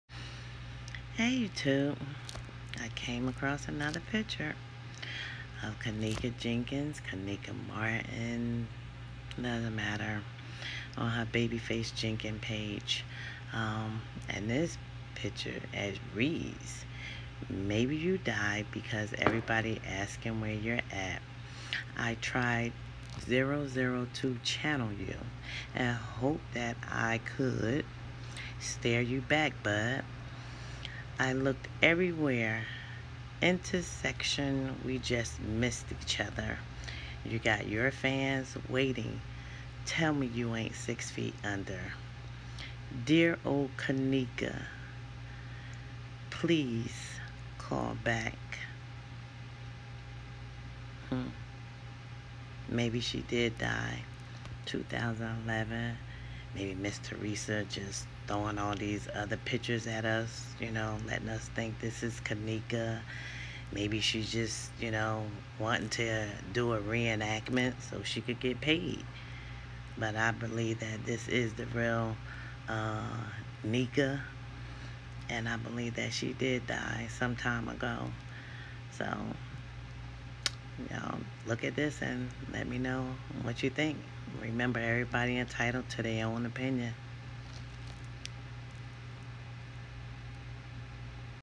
Did Kenneka died before 2017
Everybody is entitled to there own opinion I know some of you Youtubers gonna say that’s not true but this picture explain everything to me. Like I said and I’m gonna say it again it’s 2 Kenneka’s whether y’all wanna hear it or not, it was an reenactment. You decided because NEITHER ONE OF US KNEW THIS GIRL. UNLESS YOU KNEW HER THEN YOU EXPLAIN WHAT HAPPEN TO KENNEKA JENKINS MARTIN ( J4K)